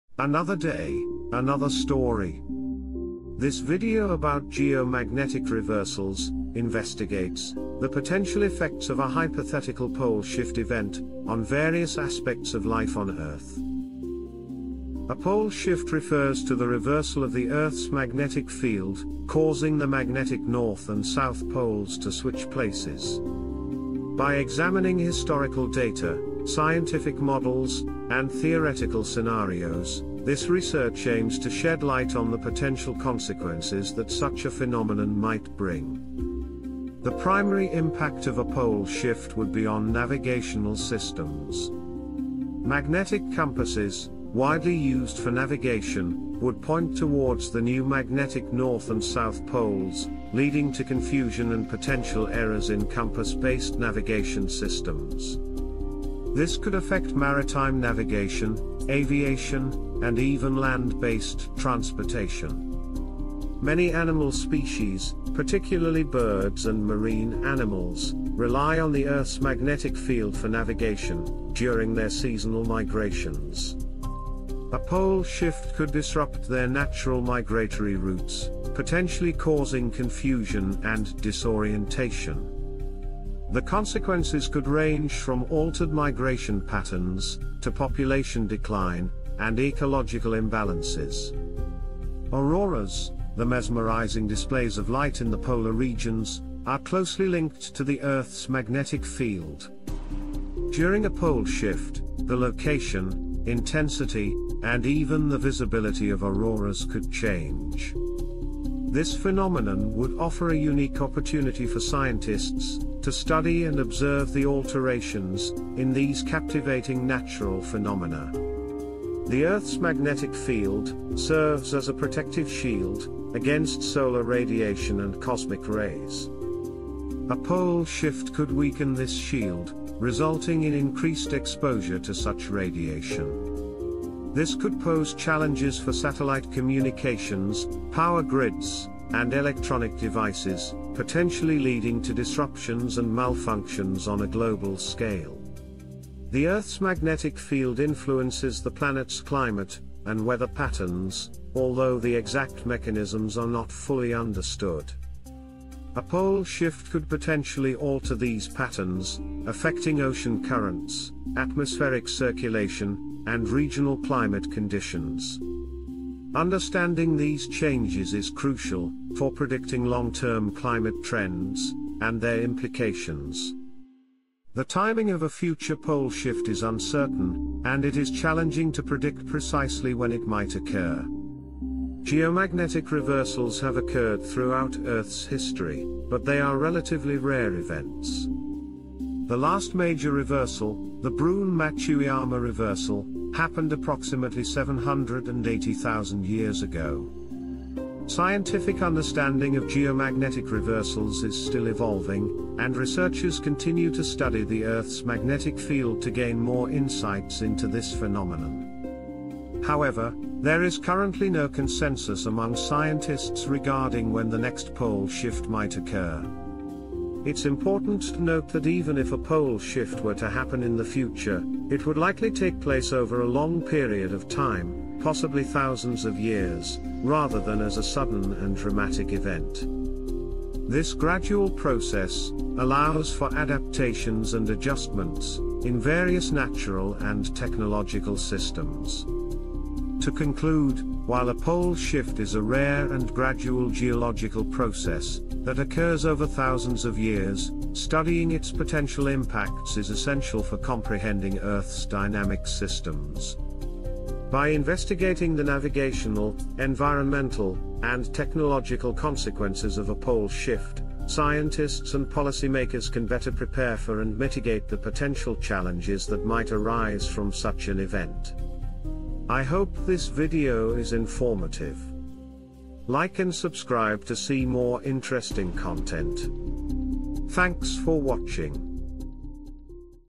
Geomagnetic Reversal: Exploring the Impacts of Earth's Pole Shift
A pole shift refers to the reversal of the Earth's magnetic field, causing the magnetic North and South poles to switch places.

By examining historical data, scientific models, and theoretical scenarios, this research aims to shed light on the potential consequences that such a phenomenon might bring.